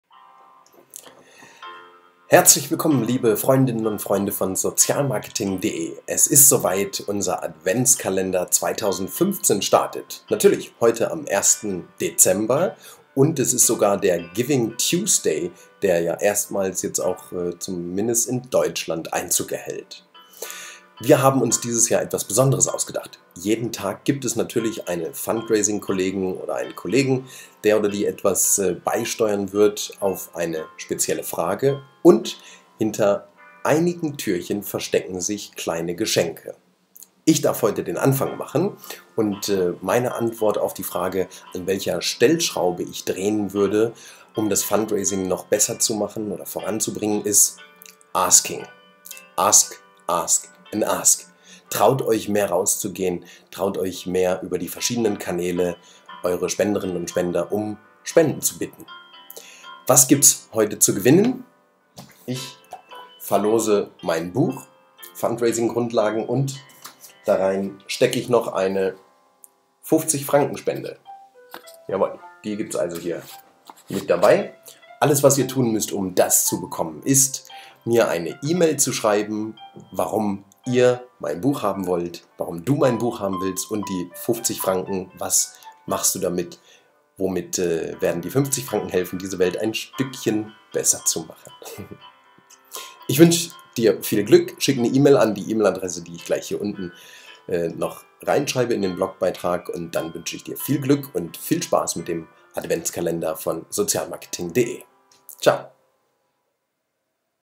FUNDCAST #120 - sozialmarketing.de-Adventskalender 2015: 1. Dezember
Im Adventskalender 2015 von sozialmarketing.de beantwortet jeden Tag eine Fundraising-Kollegin oder ein -Kollege, an welcher Stellschraube er oder sie drehen würde, um das Fundraising voran zu bringen. Jan Uekermann macht den Start! ...und nicht vergessen, jeden Tag dabei zu sein, wenn ein Türchen geöffnet wird, denn manchmal gibt es auch was zu gewinnen!!!
- dort gibt es auch die E-Mail-Adresse, unter der am 1. Dezember 2015 am Gewinnspiel teilgenommen werden kann. Die Teilnahmebedingungen sind hier zu finden